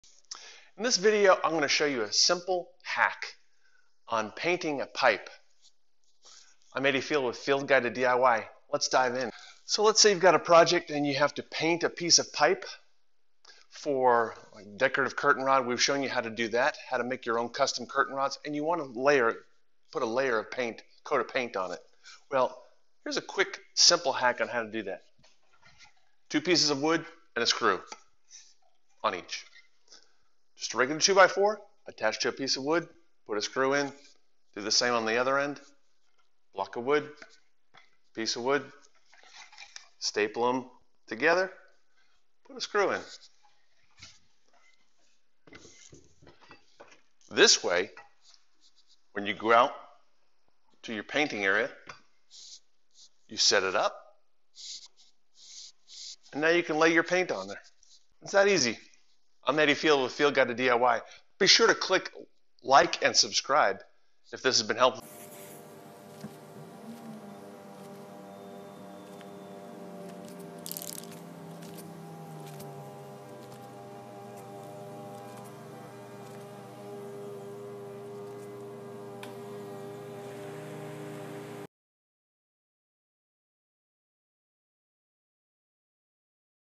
Simple Hack to Easily Paint Pipe, Rods, or Poles
Here is a simple hack or tip to quickly and easily paint pipe, rods, or poles.  We recently used this hack in making our own 13 foot custom curtain rod shown